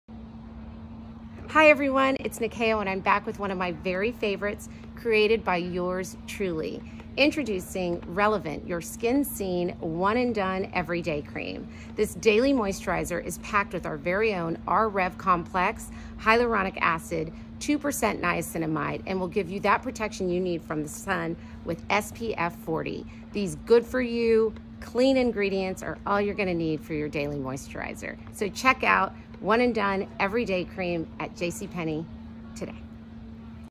Nyakio Grieco's Favorite Moisturizer for Summer | JCPenney
Created by Nyakio Grieco herself, she recommends to try Relevant One And Done Everyday Cream. This daily moisturizer is packed with ingredients to leave your skin smooth while protecting your skin from the sun with SPF 40.

Nyakio Grieco recommends to try this daily moisturizer, Relevant One And Done Everyday Cream.